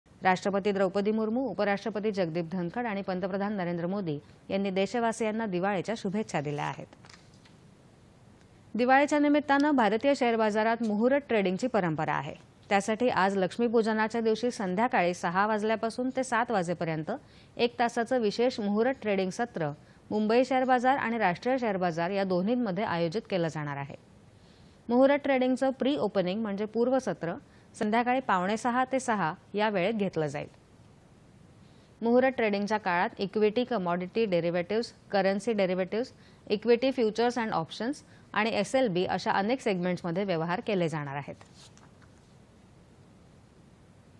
शेअर बाजारात सहा ते सात या वेळेत मुहूर्ताच्या व्यवहारांचं विशेष सत्राचे आयोजन
दि. ०१.११.२०२४ :

दिवाळीच्या निमित्ताने भारतीय शेअर बाजारात मुहूरत ट्रेडिंगची परंपरा आहे. त्यासाठी आज लक्ष्मीपूजनाच्या दिवशी संध्याकाळी सहा वाजल्यापासून ते सात वाजेपर्यंत एक तासाचं विशेष मुहूरत ट्रेडिंग सत्र मुंबई शेअर बाजार आणि राष्ट्रीय शेअर बाजार या दोन्हींमध्ये आयोजित केलं जाणार आहे. मुहूरत ट्रेडिंगचं प्रि-ओपनिंग म्हणजे पूर्वसत्र संध्याकाळी पावणेसहा ते सहा वाजेपर्यंत घेतलं जाईल. मुहूरत ट्रेडिंगच्या काळात इक्विटी, कमॉडिटी डेरीव्हेटीव्ह्ज, करन्सी डेरीव्हेटीव्ह्ज, इक्विटी फ्युचर्स अँड ऑप्शन्स आणि एसएलबी अशा अनेक सेगमेंट्समध्ये व्यवहार केले जातील.

8.30am | 11.00am | 1.00pm | 4.30pm | 7.00pm | 9.30pm